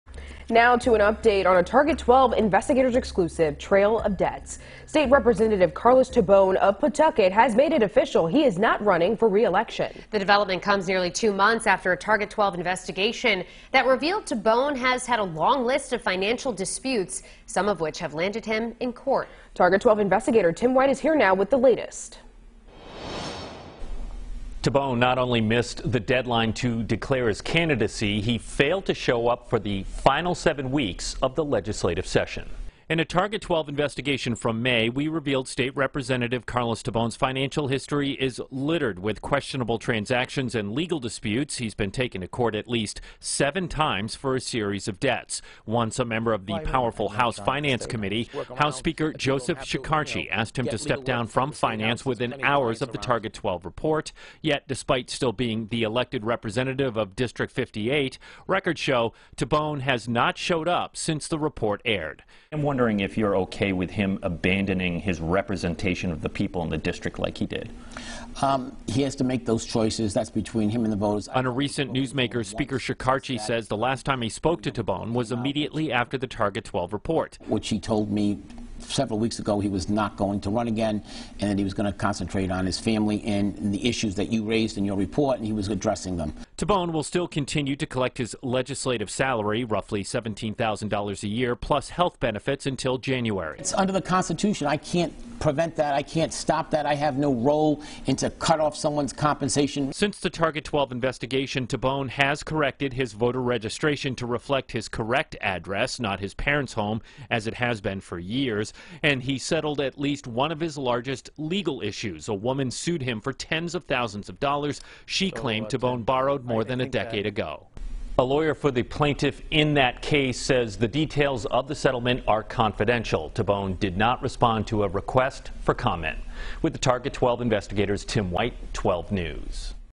McKee signs order to protect abortion providers, patients
Gov. Dan McKee signed an executive order Tuesday protecting abortion reproductive health care providers and patients statewide.